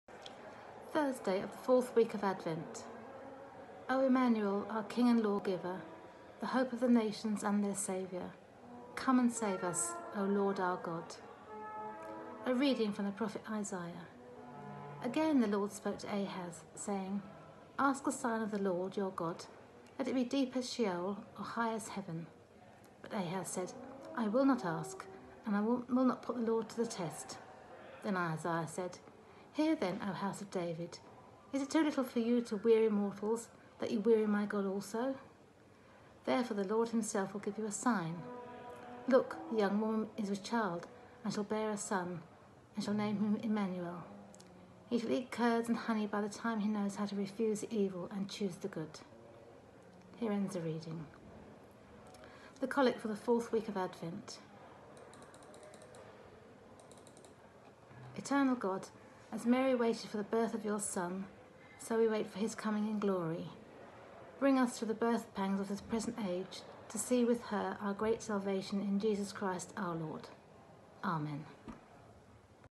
Advent Reading #22 Denise Mead
Throughout Advent members of the Clergy and Congregation of Croydon Minster have been sending readings and Prayers. In our  last reading, Denise Mead reads from Isiah 7:10.